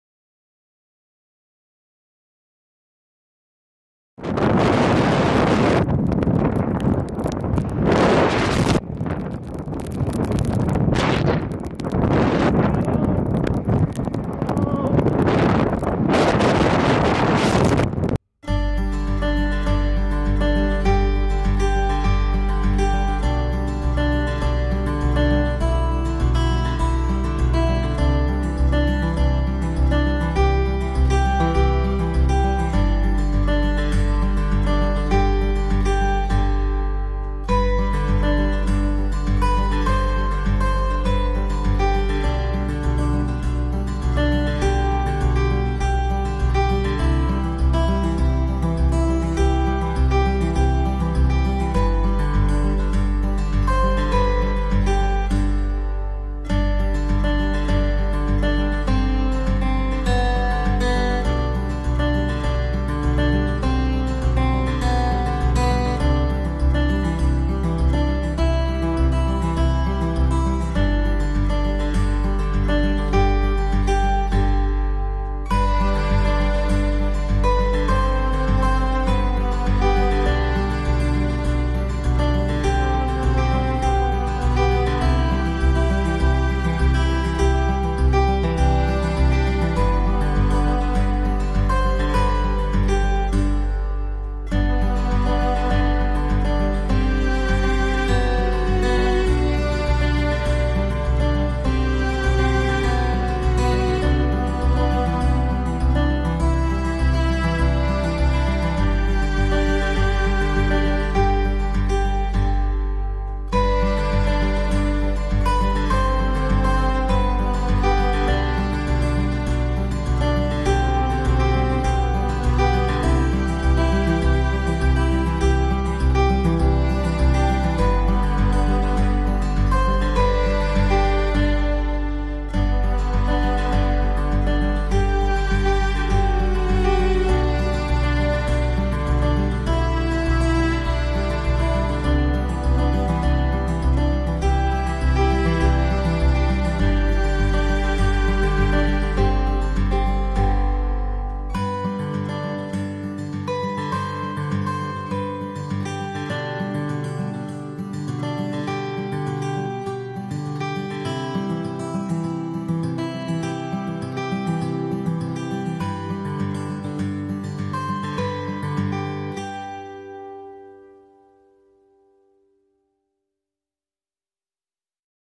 Walk up Knott Rigg and Ard Knot 30/05/2017
wet walk up Walk up Knott Rigg and Ard Knot in the lake district on the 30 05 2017